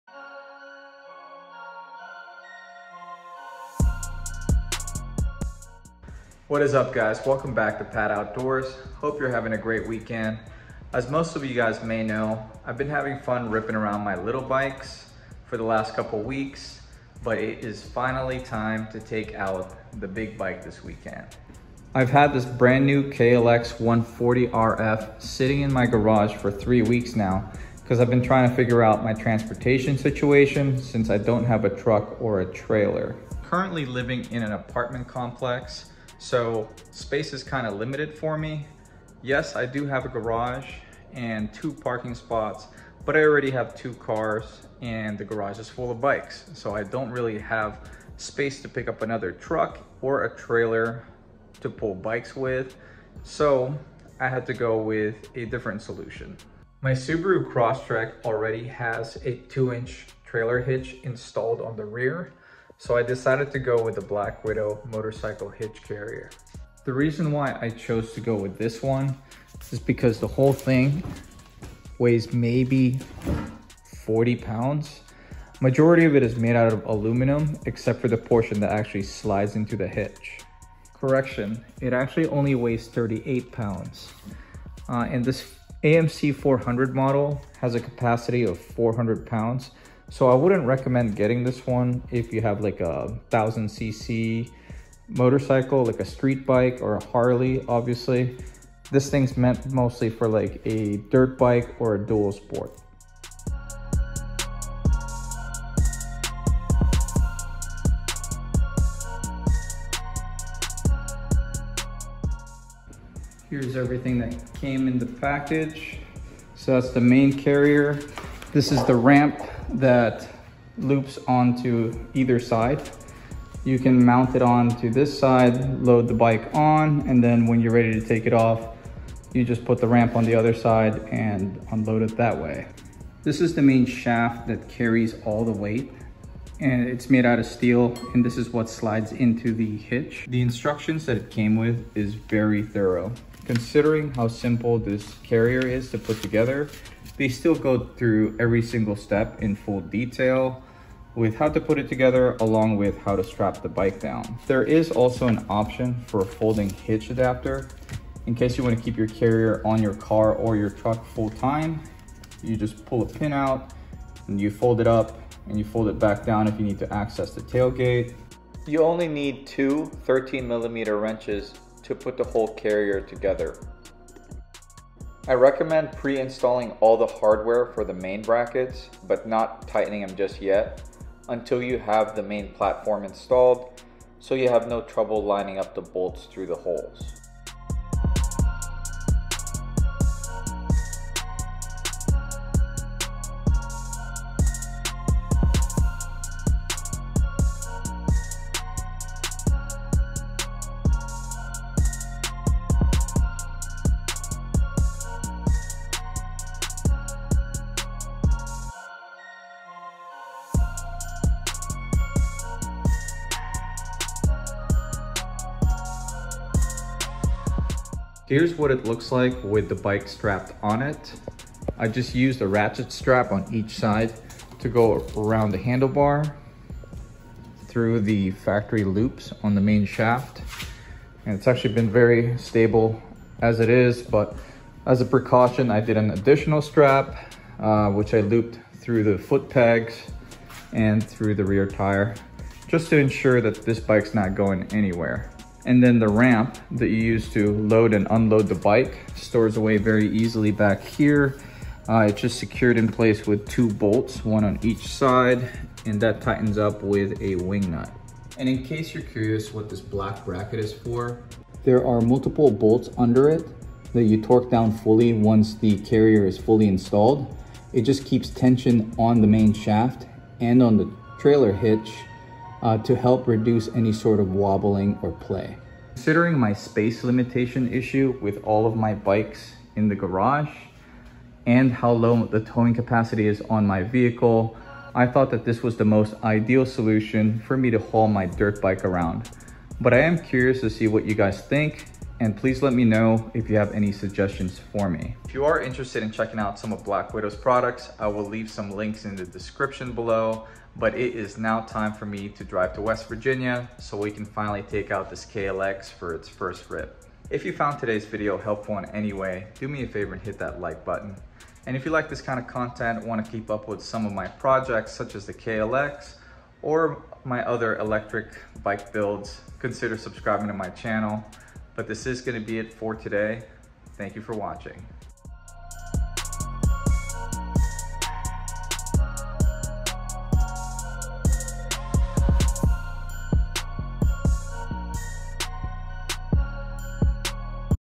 Black Widow AMC-400 Hitch-Mounted Aluminum Motorcycle Carrier | Unboxing Assembly Review
Finally got a way to haul my new Kawasaki KLX dirt bike around with our Subaru. No need to buy a truck and a trailer! Here's the one I used

AMC-400-F (Folding)
AMC-400L (Long Ramp)
MCC-500 (Steel)
MCC-500-F (Folding)
MCC-600 (Heavy Duty)